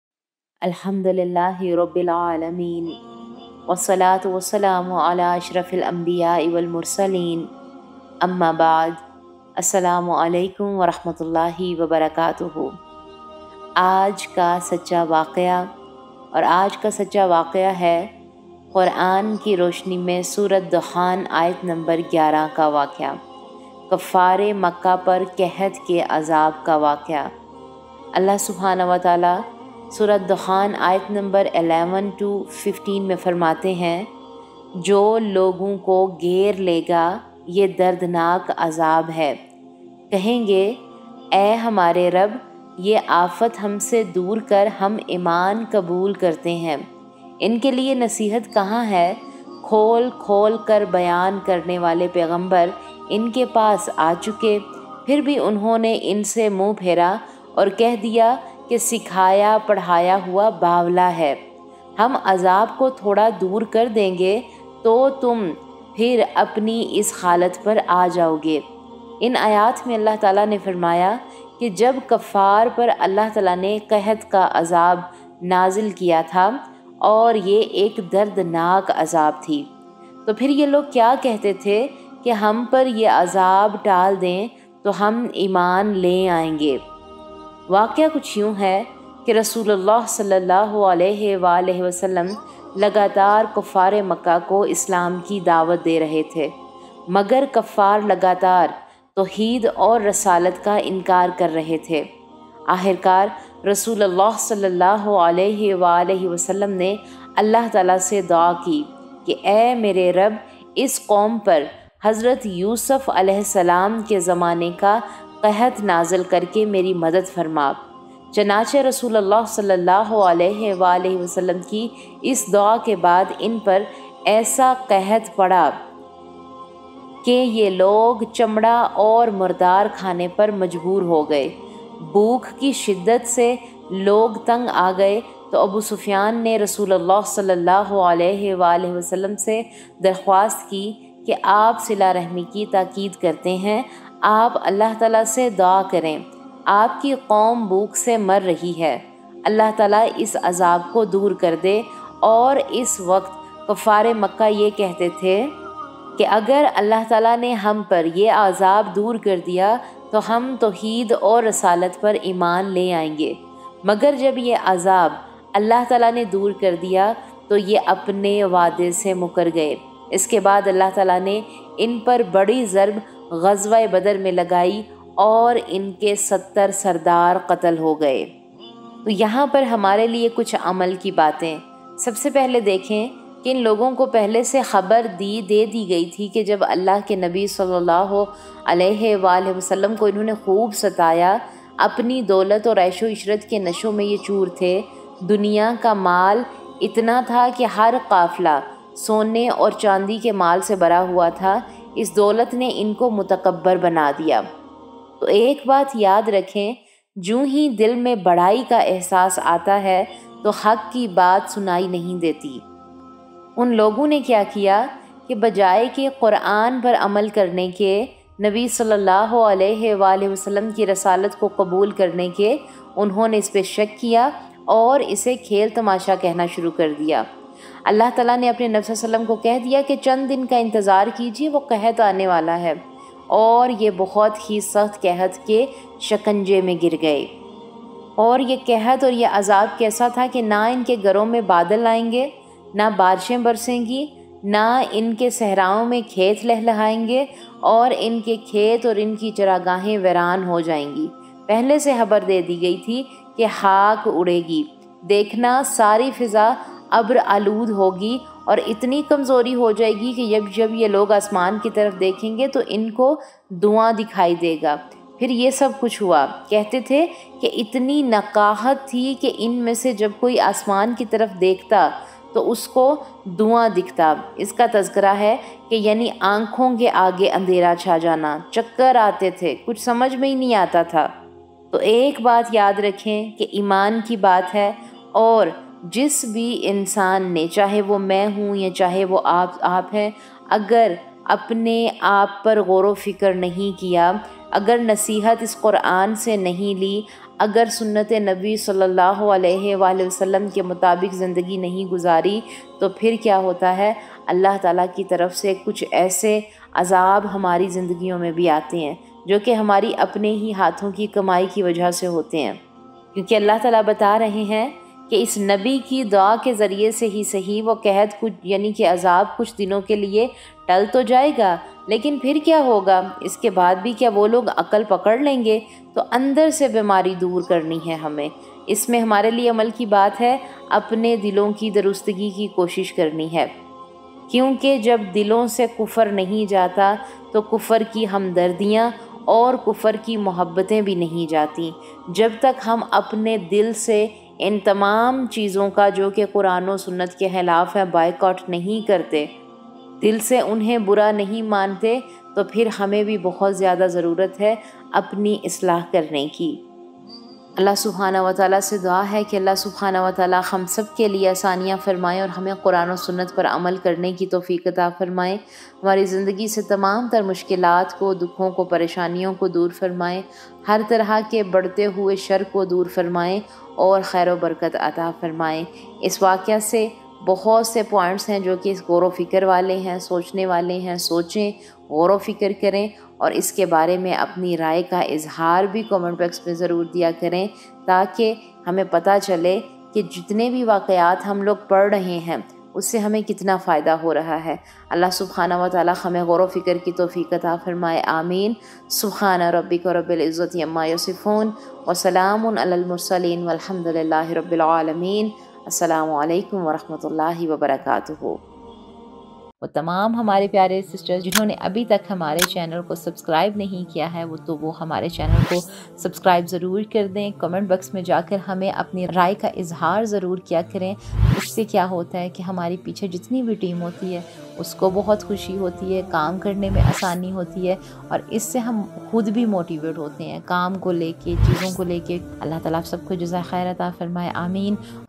Surah Ad Dukhan Ayat 11 ka waqia | کفار مکہ پر قحط کا عذاب elearningquran
Surah Ad Dukhan Ayat 11 ka waqia | کفار مکہ  پر قحط کا عذاب